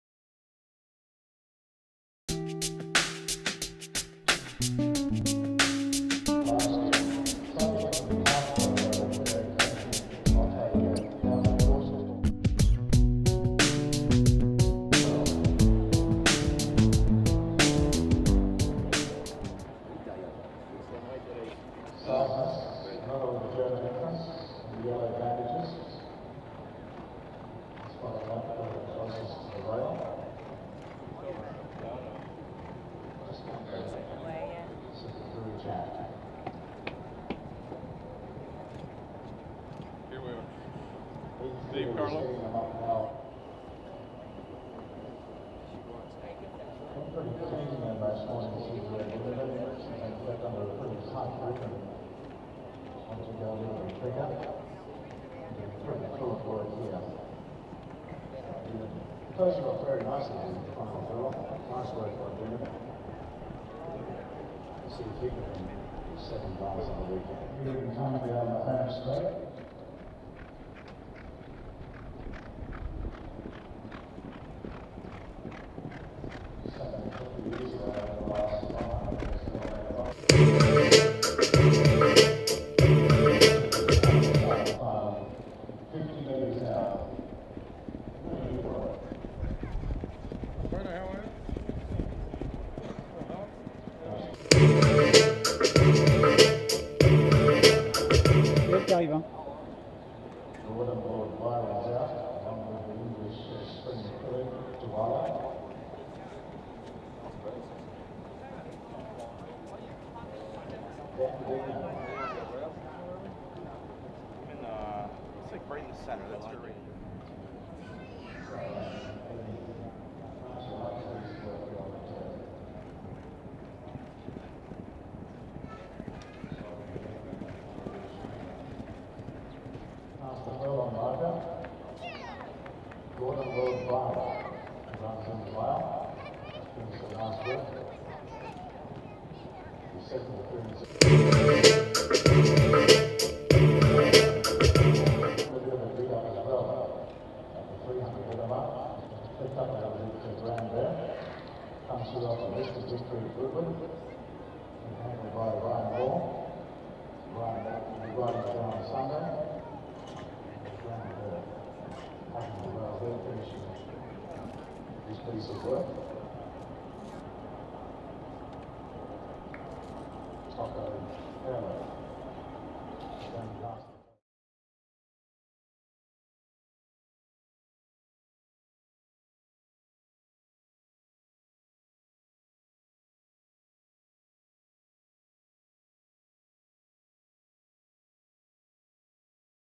Les galops de vendredi des chevaux engagés dans les grandes épreuves à Sha Tin - A2TURF.COM
Vendredi matin sur l'hippodrome de Sha Tin travaillaient plusieurs chevaux qui vont prendre part aux grandes courses de dimanche.
Parmi eux MOONLIGHT CLOUD et bien d'autres...